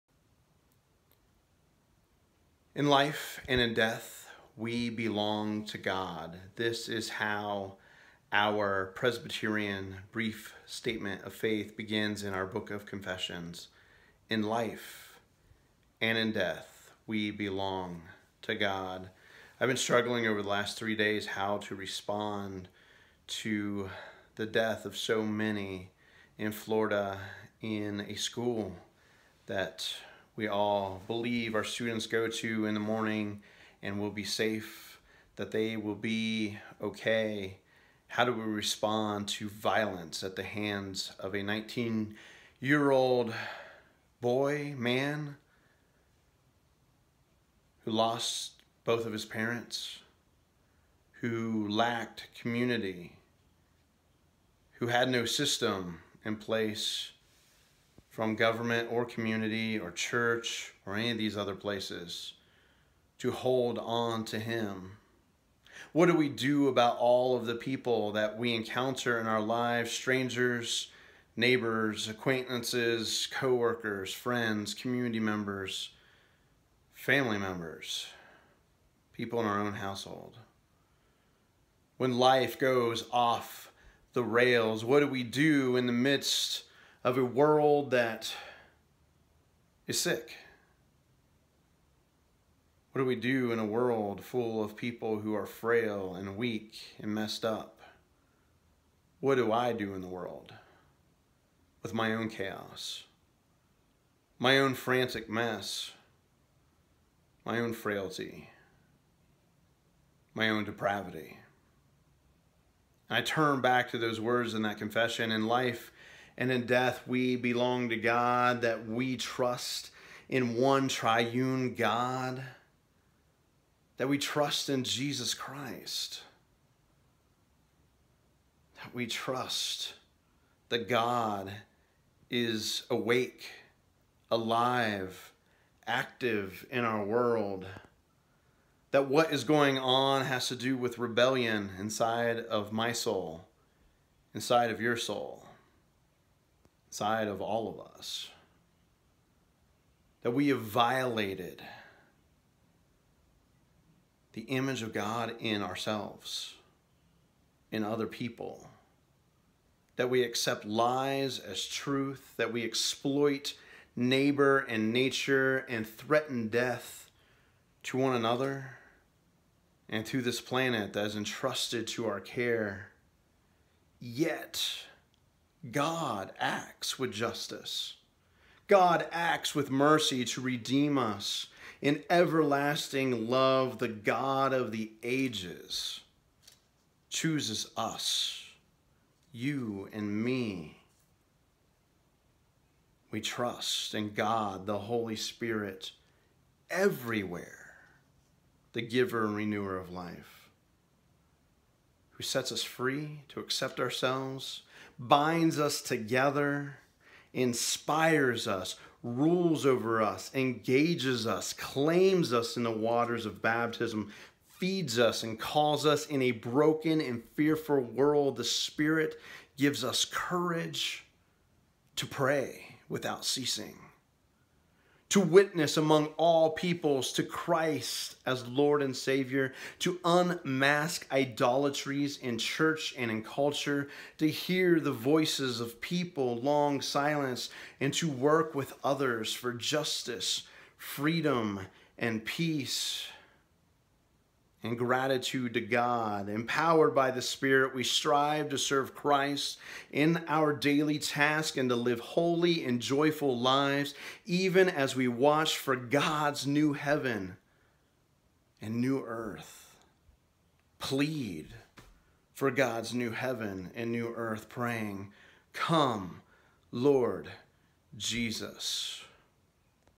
In Life and In Death We Belong To God... Come Lord Jesus
How can we respond to a massacre?  What does our faith drive us to do?  What does it mean to be faithful Christians, faithful Presbyterians, in the midst of violent tragedy?  Pastor Justin responds using the Presbyterian Book of Confessions - "in life and in death we belong to God... praying come Lord Jesus!"